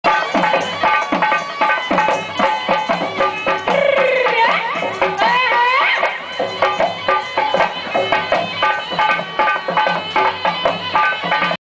Rajasthani Folk Dancers
A glimpse of Rajasthani Folk Dance Kalbeliya.